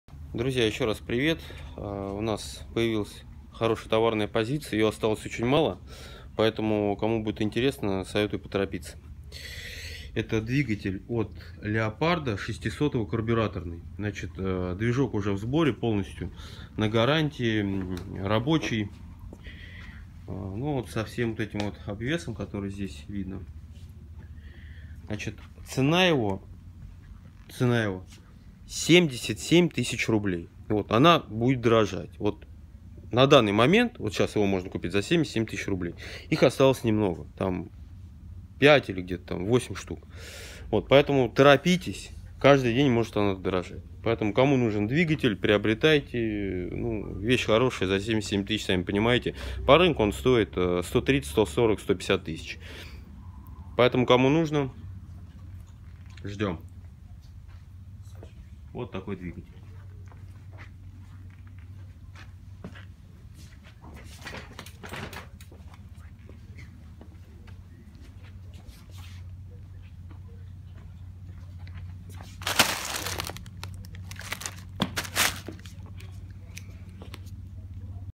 Двигатель от Леопард 600 за 77000₽
Распродажа двигателей от квадроцикла леопард 600 карбюраторный за 77000₽. Партия ограничена возможно подорожание торопитесь